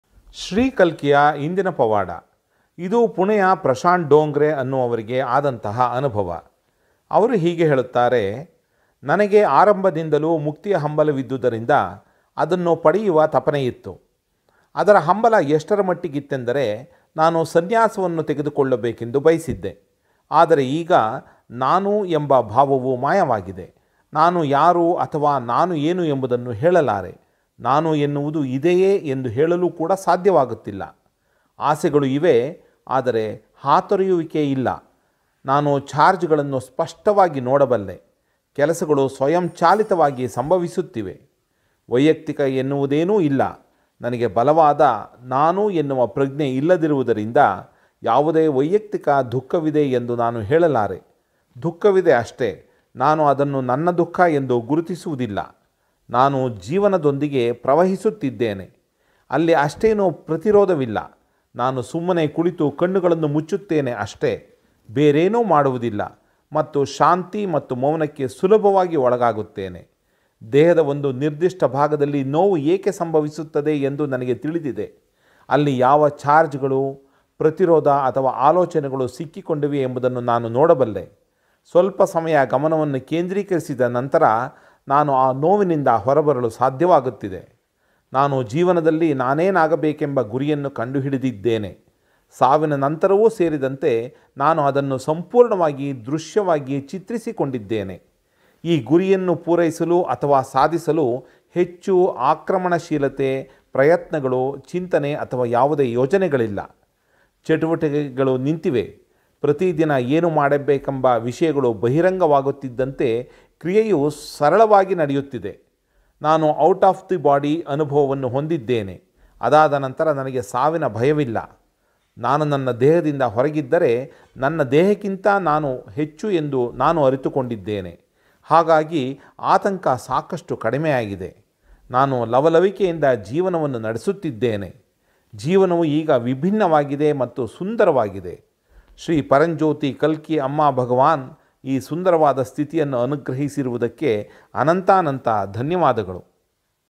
21/02. ನಾನು ಜೀವನದೊಂದಿಗೆ ಪ್ರವಹಿಸುತ್ತಿದ್ದೇನೆ. ಅಲ್ಲಿ ಅಷ್ಟೇನೂ ಪ್ರತಿರೋಧವಿಲ್ಲ.
ನಾನು ಜೀವನದಲ್ಲಿ ಏನಾಗಬೇಕೆಂಬ ಗುರಿಯನ್ನು ಕಂಡು ಹಿಡಿದಿದ್ದೇನೆ. ಸಾವಿನ ನಂತರವೂ ಸೇರಿದಂತೆ ನಾನು ಅದನ್ನು ಸಂಪೂರ್ಣವಾಗಿ ದೃಶ್ಯವಾಗಿ ಚಿತ್ರಿಸಿಕೊಂಡಿದ್ದೇನೆ. ಈ ಗುರಿಯನ್ನು ಪೂರೈಸಲು ಅಥವಾ ಸಾಧಿಸಲು ಹೆಚ್ಚು ಆಕ್ರಮಣಶೀಲತೆ, ಪ್ರಯತ್ನಗಳು, ಚಿಂತನೆ ಅಥವಾ ಯಾವುದೇ ಯೋಜನೆಗಳಿಲ್ಲ. ಶ್ರೀ ಪರಂಜ್ಯೋತಿ ಕಲ್ಕಿ ಅಮ್ಮಭಗವಾನ್! ಈ ಸುಂದರವಾದ ಸ್ಥಿತಿಯನ್ನು ಅನುಗ್ರಹಿಸಿರುವುದಕ್ಕೆ ನಿಮಗೆ ಅನಂತಾನಂತ ಧನ್ಯವಾದಗಳು.